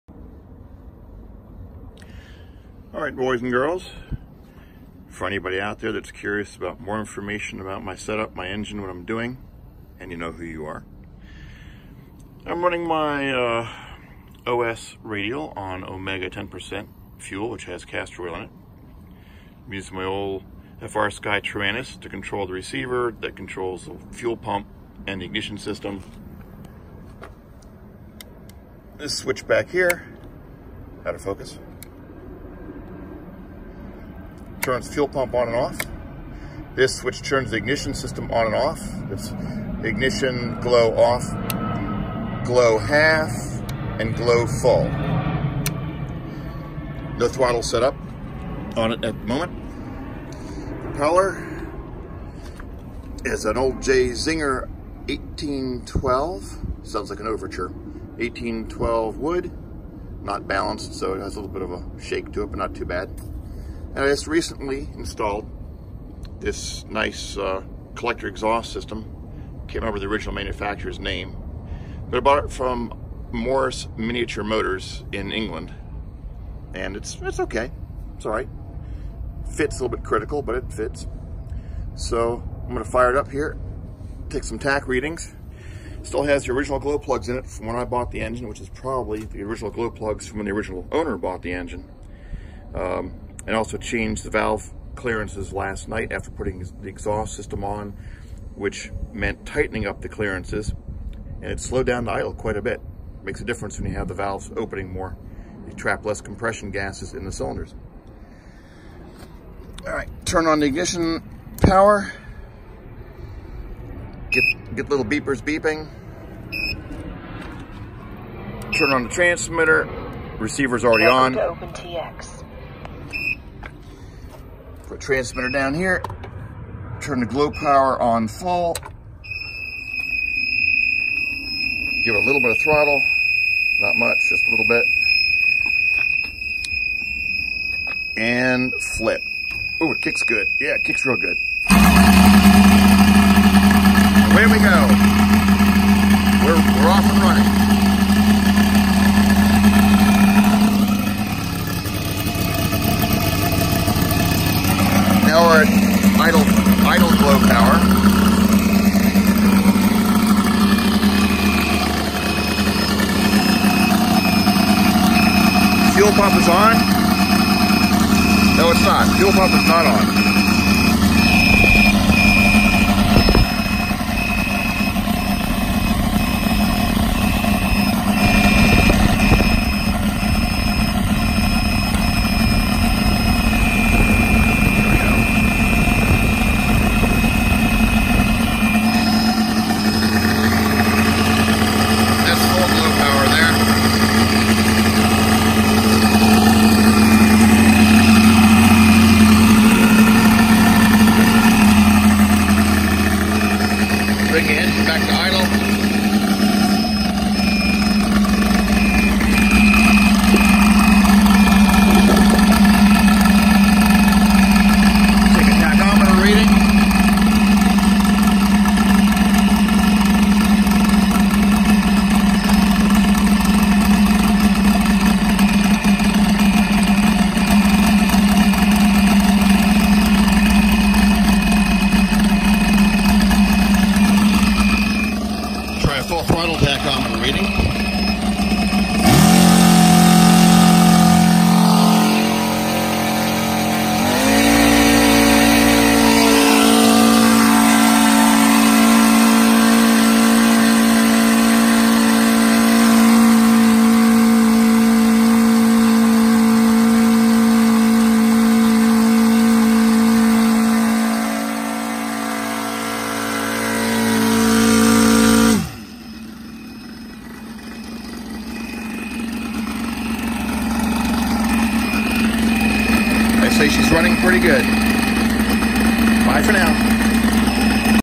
More O.S. FR5-300 Radial Engine Testing
New collector ring exhaust, adjusted valves, new main and header fuel tanks, performance information and more...